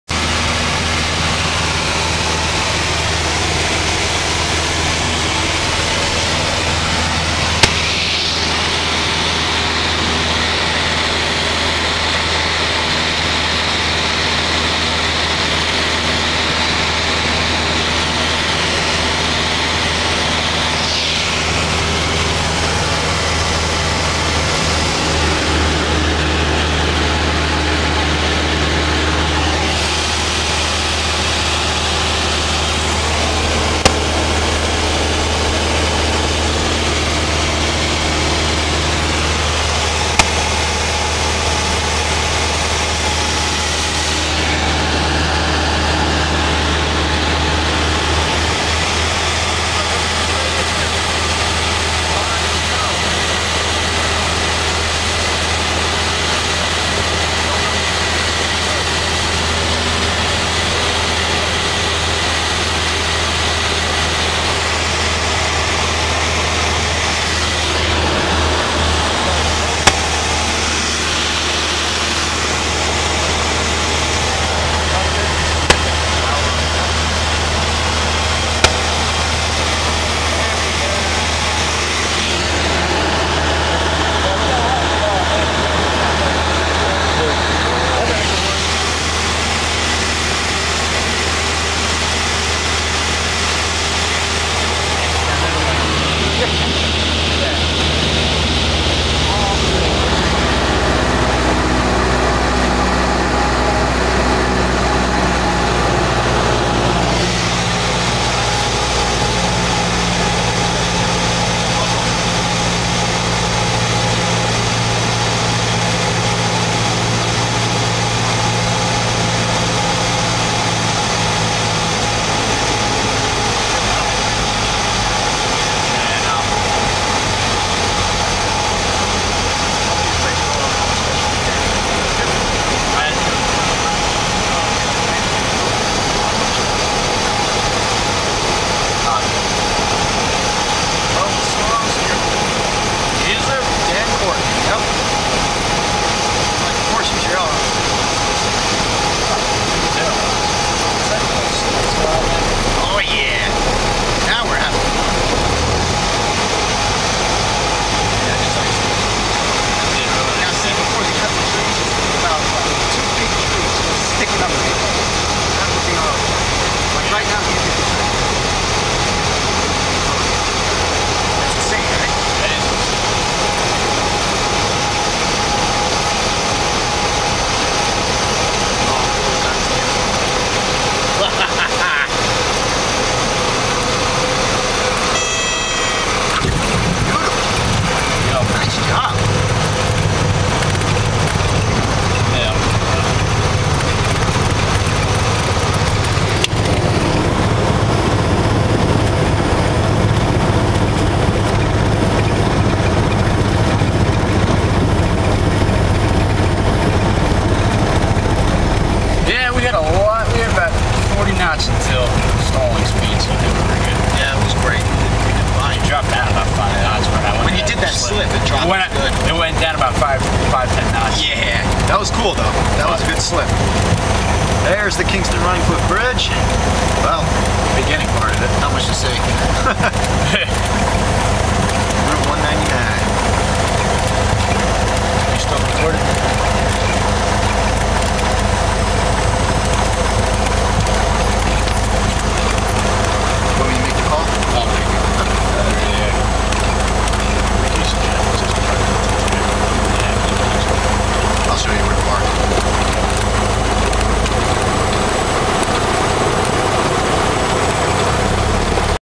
Flying Around Upstate New York C-172
A buddy of mine had just earned his private pilot license and was ready to go flying. A month later we did just that. Here is the result!

Plane C-172/G